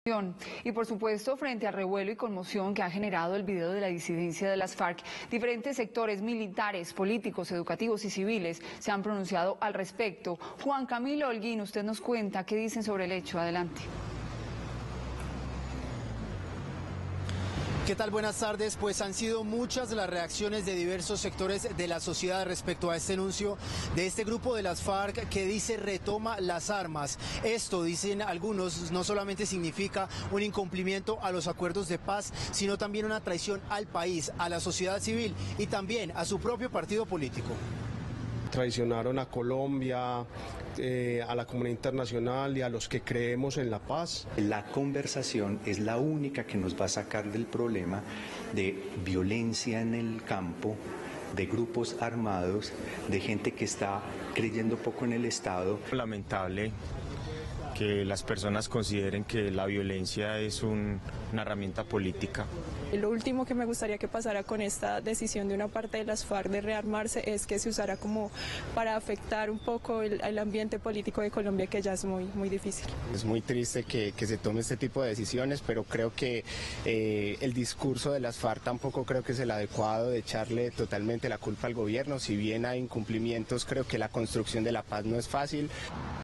Distintos sectores de la sociedad reaccionaron ante video de las Farc [Noticias] - Telemedellín
Frente al revuelo y conmoción que ha generado el video de la disidencia de las Farc, diferentes sectores políticos, educativos y civiles se pronunciaron al respecto.